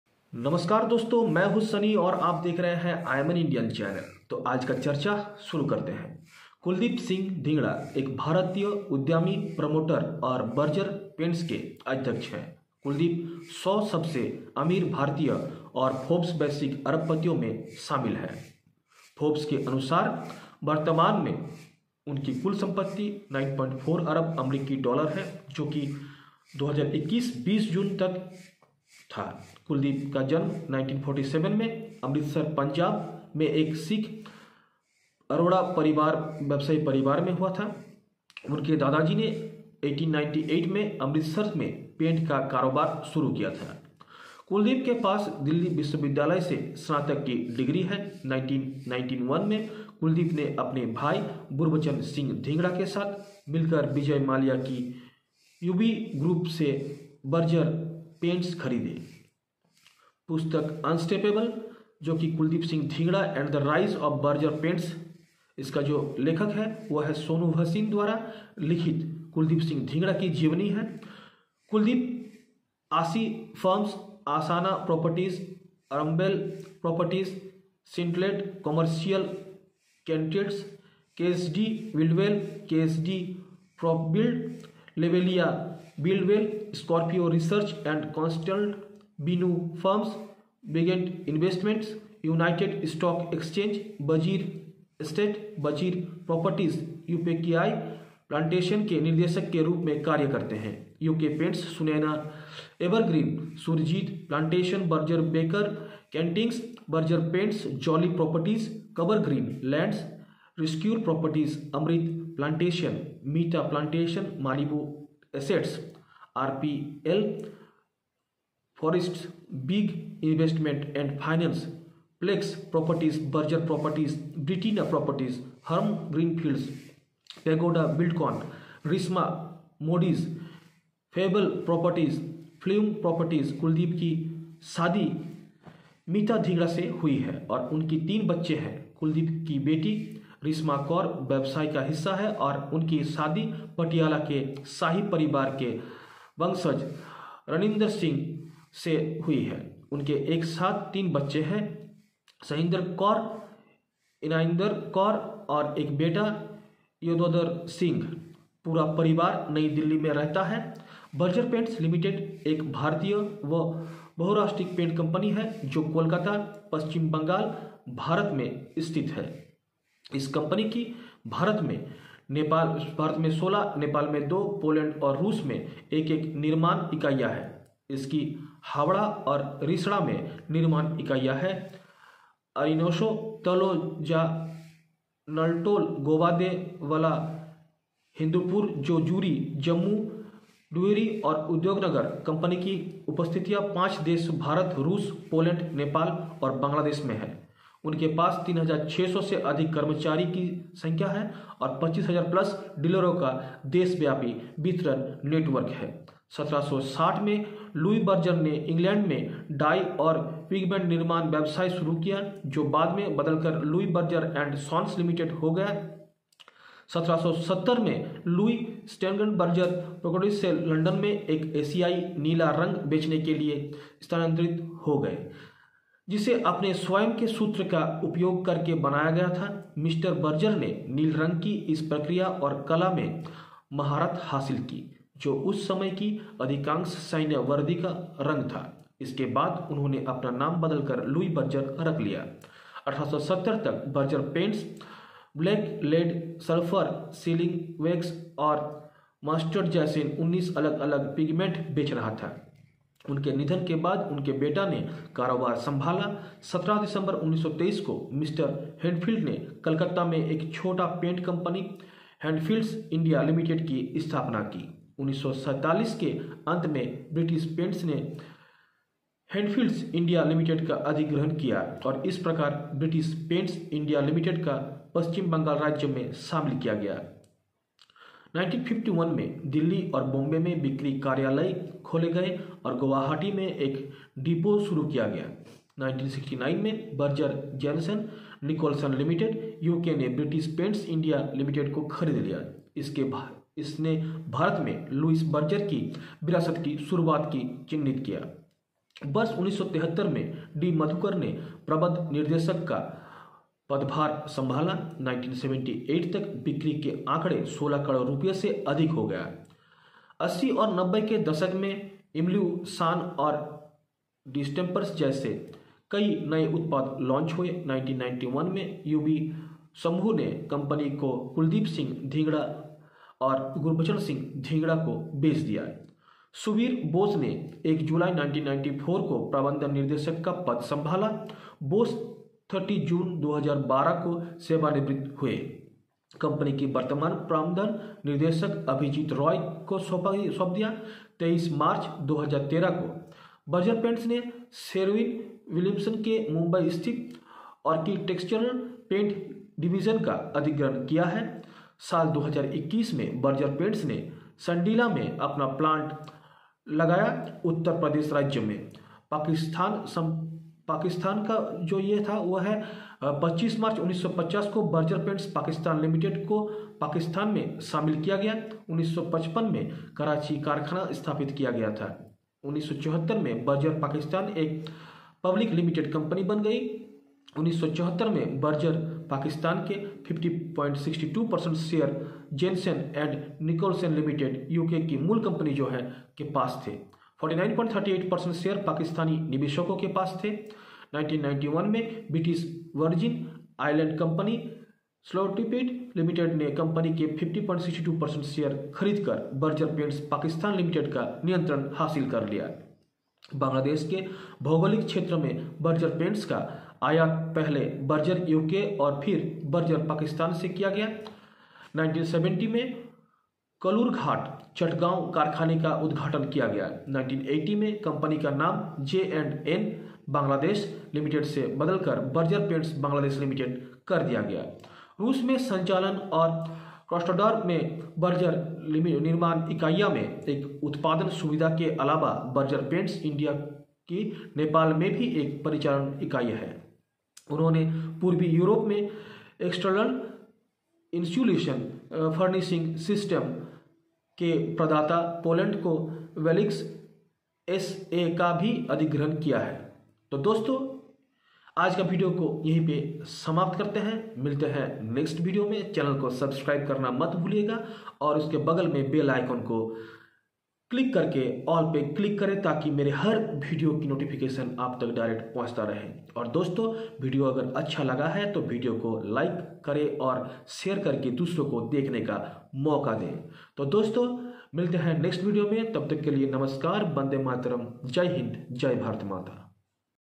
Kuldip and Gurbachan Singh Dhingra Success Story Of Berger Paints
Berger Paints
Indian multinational paint company

Berger Paints Ltd is an Indian multinational paint company, based in Kolkata, West Bengal, India. This company has 16 manufacturing units in India, 2 in Nepal, 1 each in Poland and Russia. It has manufacturing units at Howrah and Rishra, Arinso, Taloja, Naltoli, Goa, Devla, Hindupur, Jejuri, Jammu, Puducherry and Udyognagar. The company has presence in five countries – India, Russia, Poland, Nepal and Bangladesh. They have an employee strength of over 3,600 and a countrywide distribution network of 25,000+ dealers.

History
European
In 1760, Louis Berger, started a dye and pigment manufacturing business in England, which later changed to Louis Berger & Sons Limited. In 1770, Louis Steigenberger shifted from Frankfurt to London to sell a Prussian blue colour, which was made using his own formula. Mr. Berger perfected this process & art of the blue colour, which was the colour of most military uniforms of that time. He then changed his name to Lewis Berger. By 1870, Berger Paints was selling 19 different pigments such as black lead, sulphur, sealing wax and mustard. After his demise, his sons took over the business.

Indian sub-continent
On 17 December 1923, Mr. Hadfield set up Hadfield's (India) Ltd., a small paint company in Calcutta. Towards the end of 1947, British Paints acquired Hadfield's (India) Ltd and thus British Paints (India) Ltd was incorporated in the State of West Bengal. In 1951, sales offices were opened in Delhi and Bombay and a depot was started in Guwahati. In 1969, Berger Jenson Nicholson Limited, UK bought British Paints (India) Ltd. This marked the beginning of Lewis Berger's legacy in India. In the year 1973, D. Madhukar took over as the managing director. Sales figures reached over ₹16 crore by 1978. The 80s and the 90s saw the launch of many new products such as emulsions and distempers. In 1991, UB group sold the company to Kuldip Singh Dhingra (Chairman) and Gurbachan Singh Dhingra (Vice Chairman). Subir Bose took over as managing director on 1 July 1994. Bose retired on 30 June 2012, handing over the company to Abhijit Roy, the current managing director. On Mar 13, 2013, Berger Paints has acquired the Mumbai-based architectural paints division of Sherwin Williams. In the year 2021, Berger Paints set up their plant in Sandila in the state of Uttar Pradesh.

Pakistan
On 25 March 1950, Berger Paints Pakistan Limited was incorporated in Pakistan. In 1955, the Karachi factory was established. In 1974, Berger Pakistan became a public limited company. In 1974, Berger Pakistan 50.62% shares were held by Jenson & Nicholson Limited (U.K. parent company), 49.38% shares were held by Pakistani investors. In 1991, Slotrapid Limited, a British Virgin Island company, acquired control of Berger Paints Pakistan Limited by purchasing 50.62% shares of the company.

Bangladesh
In the geographical region of Bangladesh, Berger Paints were imported from Berger UK and then from Berger Pakistan. In 1970, the Kalurghat, Chittagong factory was inaugurated. In 1980, the name of the company changed from J&N (Bangladesh) Limited to Berger Paints Bangladesh Limited.

Operations
Apart from operations in Russia and a production facility at the Berger manufacturing unit at Krasnodar, Berger Paints India also has an operational unit in Nepal. They have also acquired Bolix SA of Poland, a provider of External Insulation Finishing Systems (EIFS) in Eastern Europe.